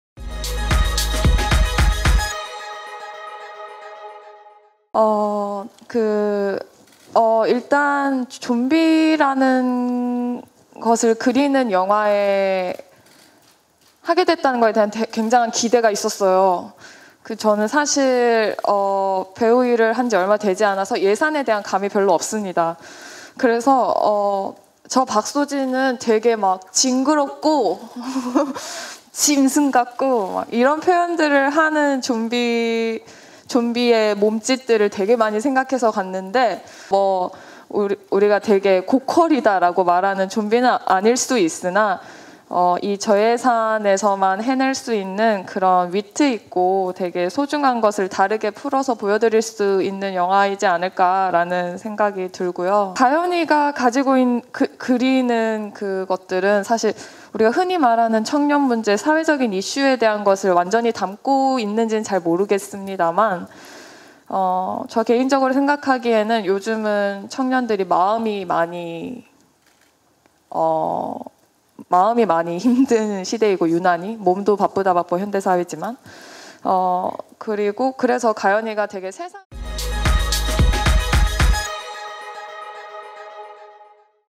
‘좀비크러쉬: 헤이리’ 박소진(PARK SO JIN) “저예산 영화에서만 보일 수 있는 위트를 담은 좀비영화”
영화 '좀비크러쉬: 헤이리' 언론시사회가 23일 오전 서울 광진구 자양동 롯데시네마 건대입구점에서 열렸다.

이날 언론시사회에는 공민정 이민지 박소진 조승구 김준식, 장현상 감독이 참석했다.

'좀비크러쉬: 헤이리'는 좀비 바이러스로 초토화된 헤이리 예술마을을 구하는 과정으로 오는 30일 개봉된다.